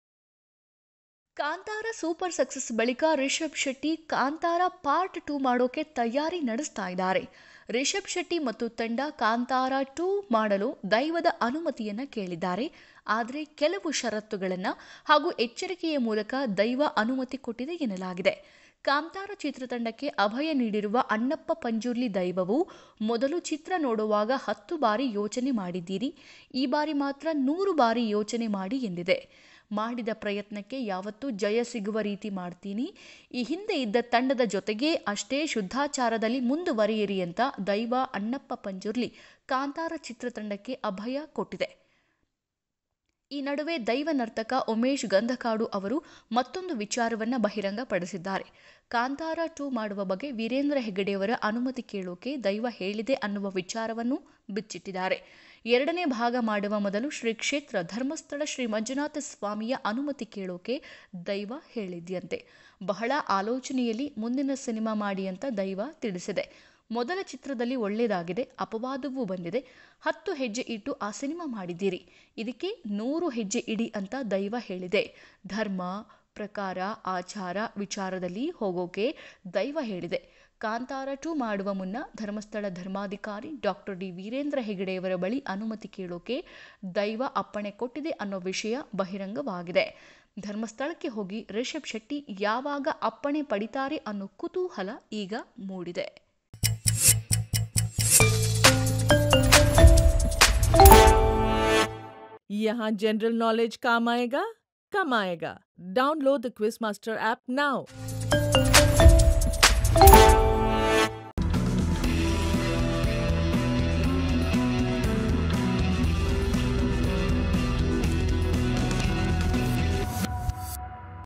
ದೈವದ ಮುಂದೆ ರಿಷಬ್ ಪ್ರಶ್ನೆ, ಧರ್ಮಸ್ಥಳದಲ್ಲಿ ಕಾಂತಾರ 2 ನಿರ್ಧಾರ | SDC WORLD KANNADA
Kantara |Rishab Shetty| SDC World Kannada Kantara | Rishab Shetty | Vijay Kiragandur | Hombale Films| SDC World Kannada